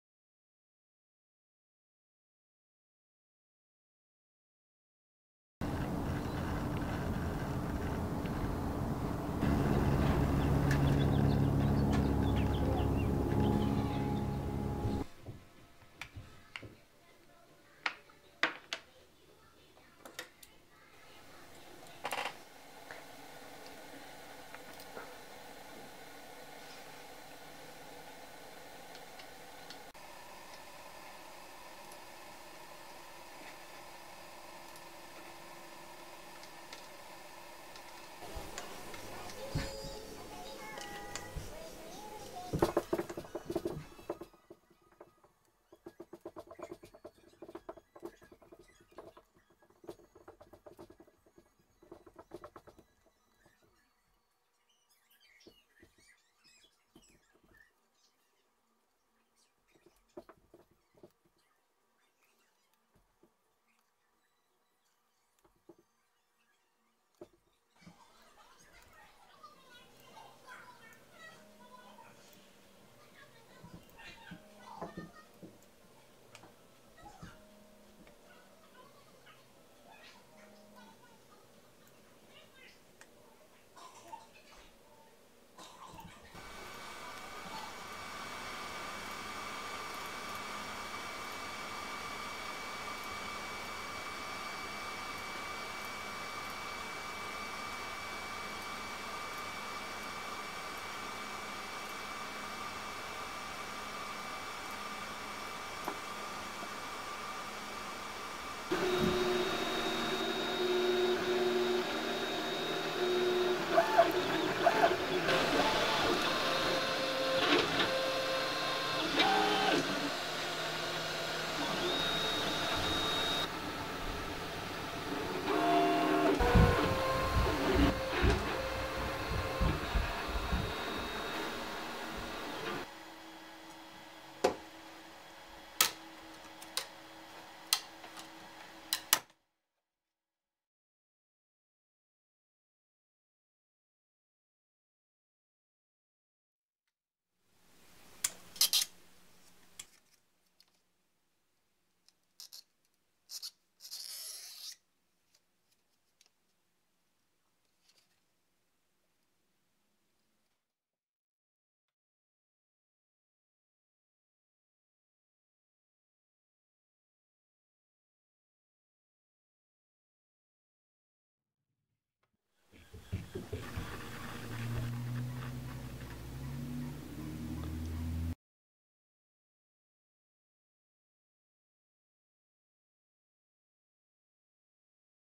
Phone Case Design and 3D Print
Video shows the steps for designing and fabricating a custom phone case on a 3D printer.  Test video.